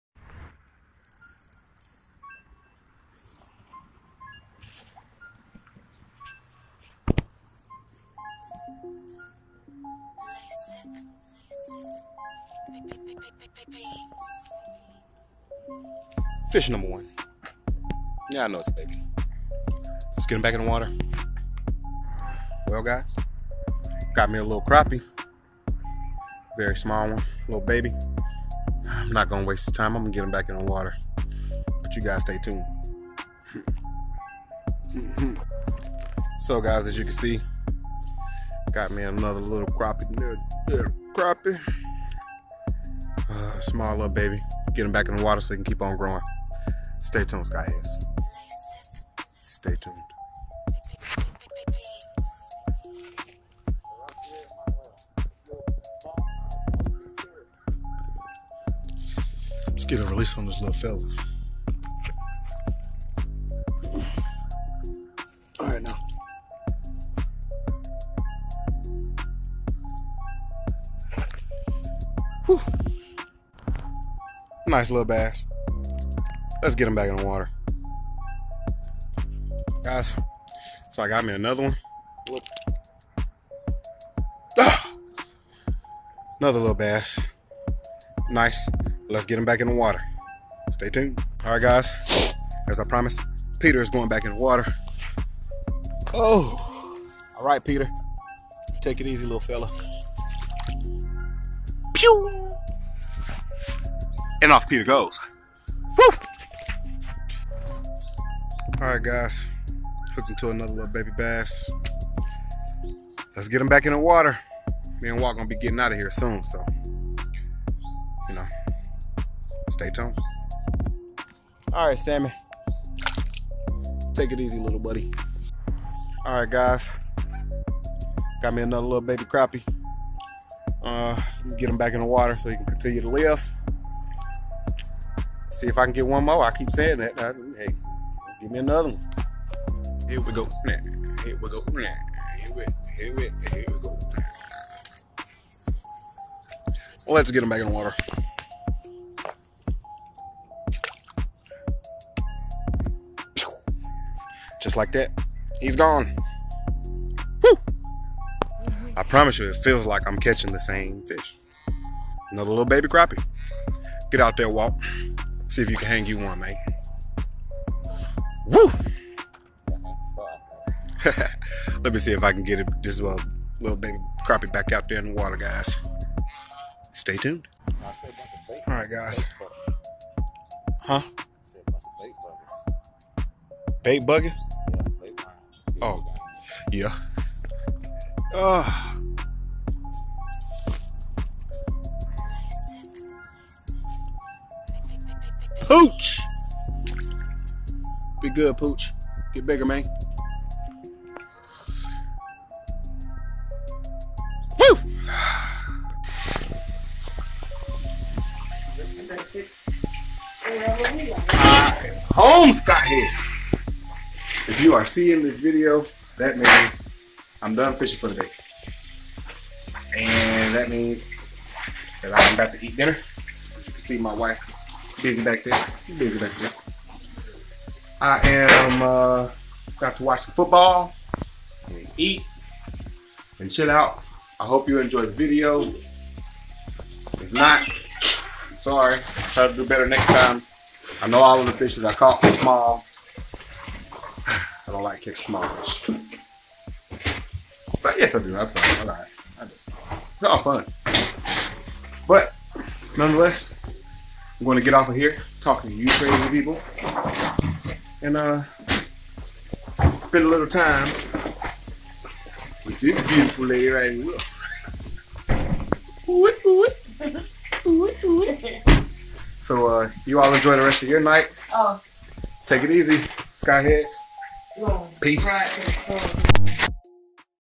Crappie fishing (caught DINKS and CRAPPIE all day )
Went fishing for a while and caught a bunch of small babies. Hope you all enjoy!